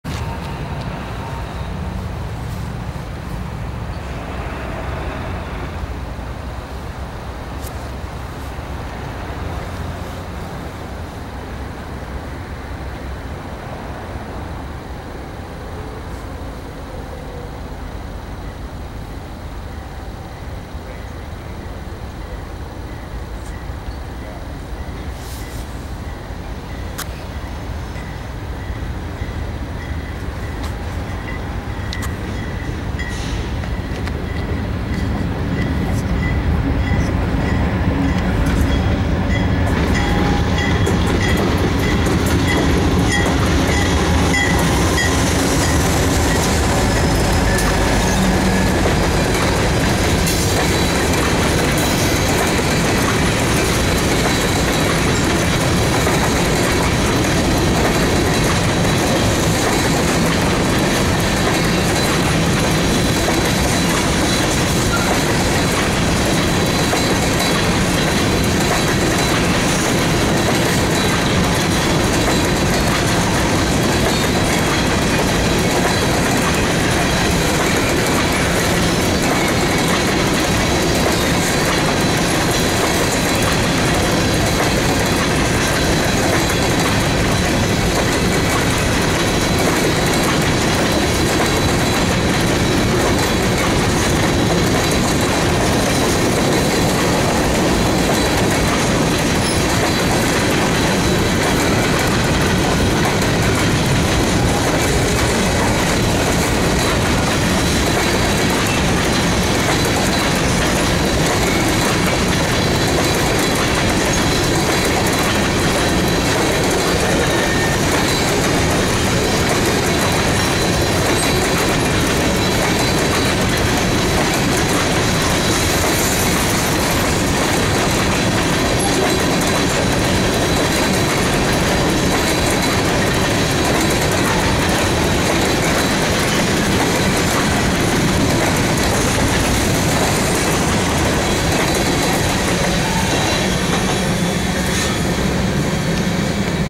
Empty Grain Train heading West by Ayer Station on a cold Sunday Night 12/30/18
Next Train to go by at Ayer Station after 28N was Empty Grain Train heading West to get the rest of the Grain Cars in Fitchburg Yard on a cold Sunday Night 12/30/18 so Train movement that Night was fantastic for being out there.  Enjoy my video everyone.